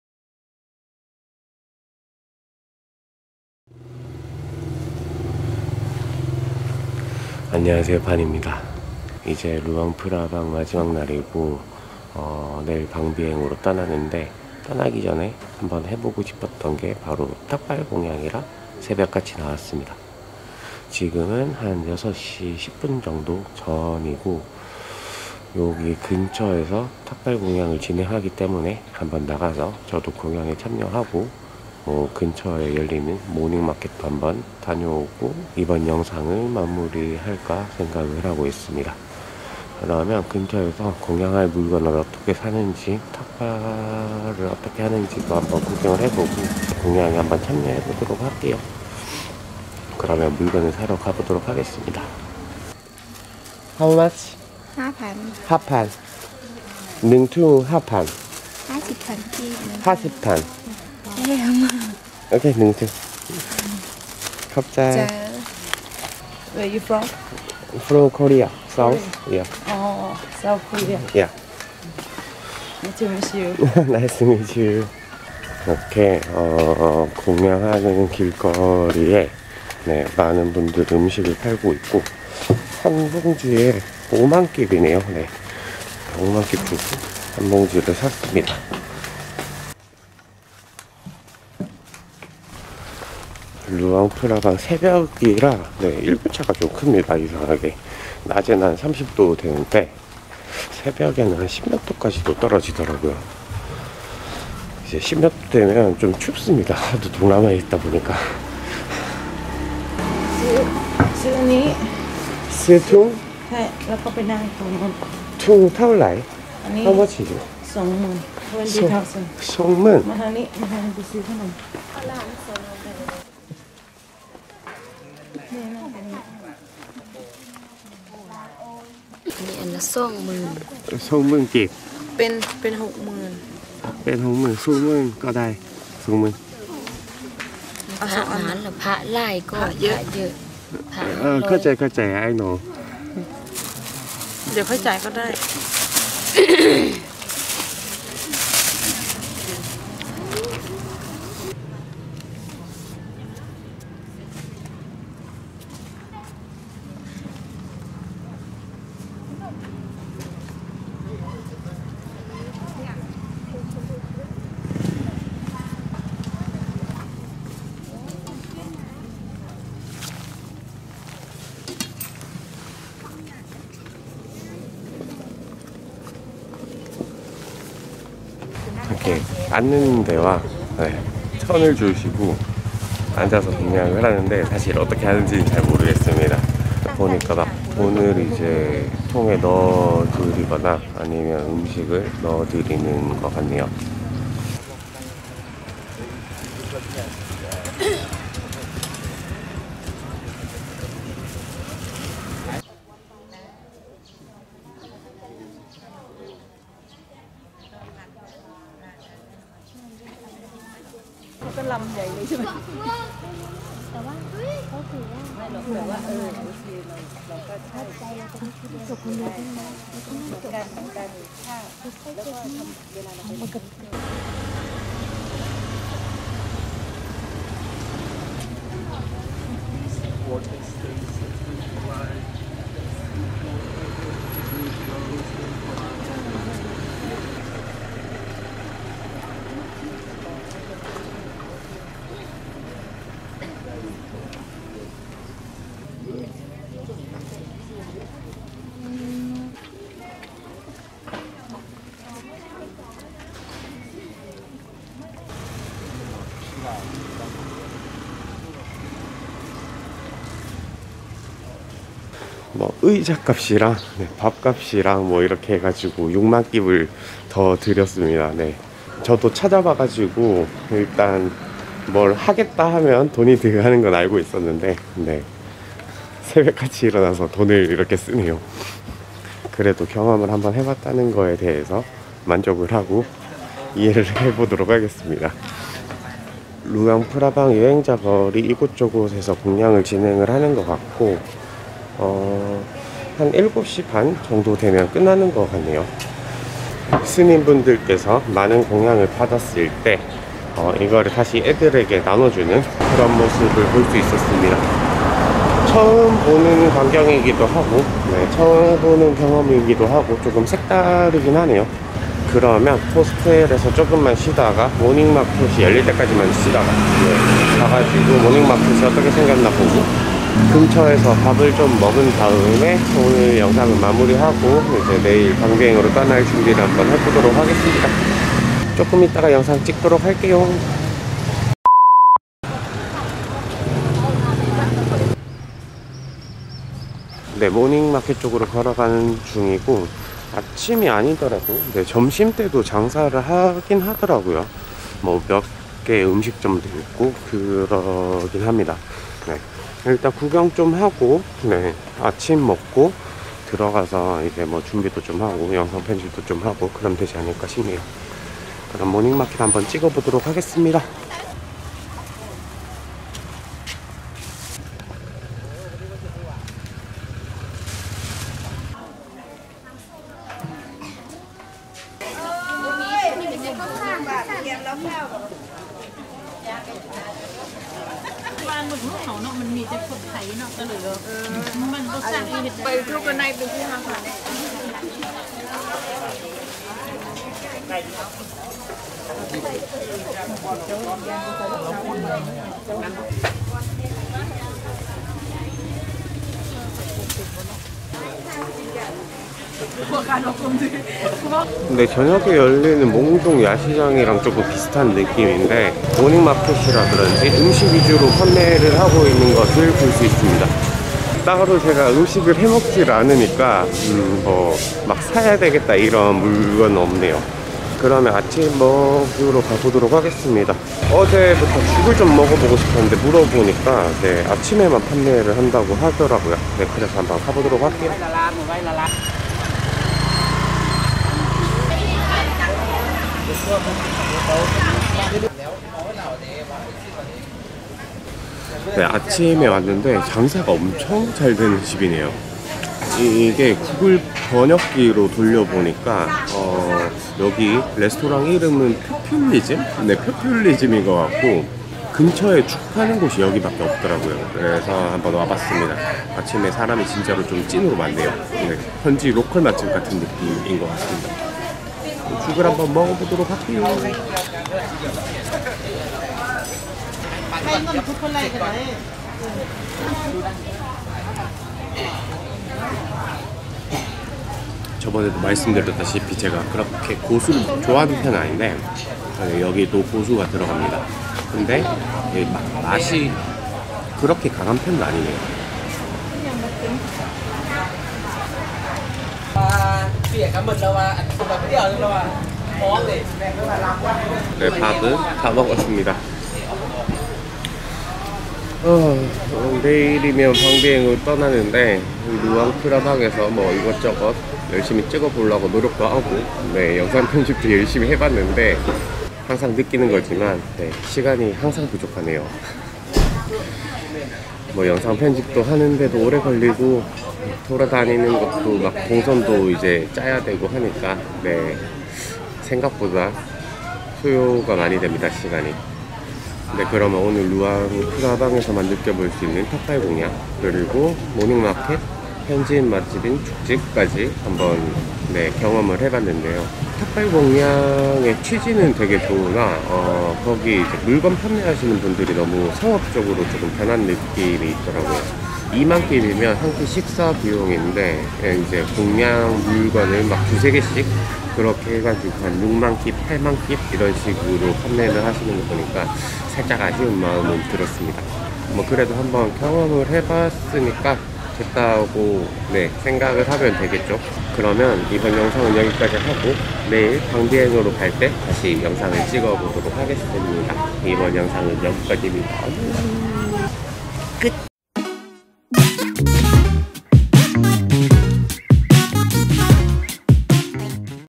ep.33 | 라오스 루앙프라방 탁발 공양 참여
라오스 루앙프라방 하면 가장 먼저 생각나는 탁발 공양!
새벽같이 일어나서 잠도 덜 깬 채로 공양에 참여
근처 모닝마켓도 둘러본 뒤 현지 맛집 레스토랑까지